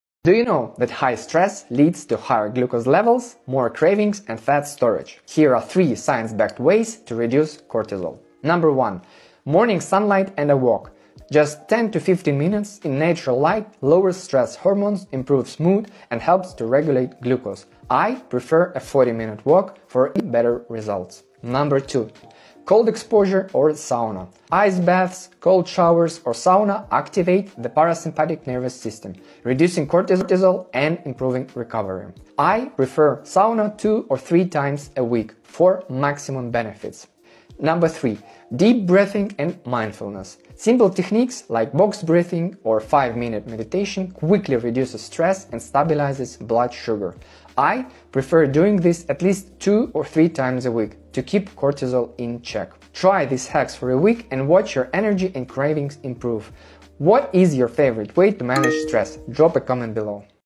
How Stress Raises Your Blood Sugar (and How to Fix It)
High Stress = Higher Glucose & More Cravings – Fix It Fast! NB! I'm not diabetic, just mindful of insulin. CGM data is personal-test foods yourself. Don't forget about calories and ingredient quality!